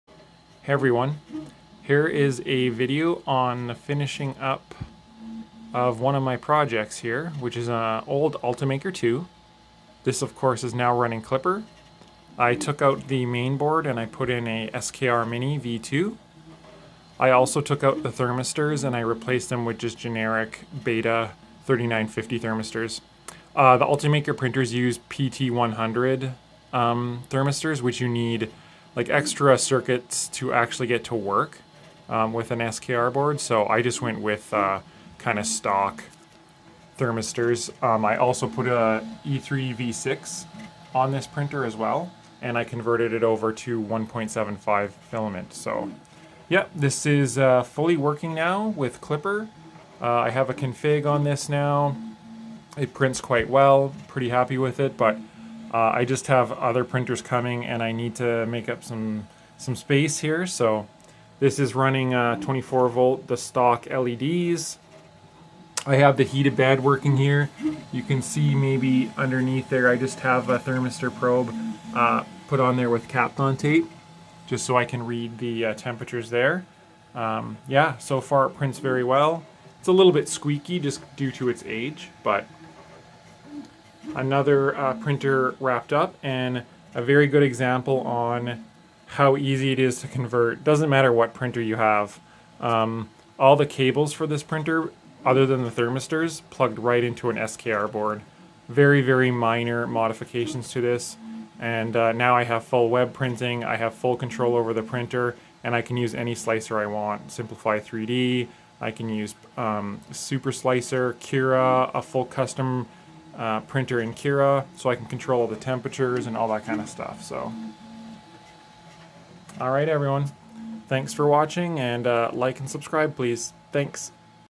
Ultimaker 2 with SKR Mini E3 and Klipper!
I have converted this Ultimaker 2 to use a SKR mini E3 v2 and Klipper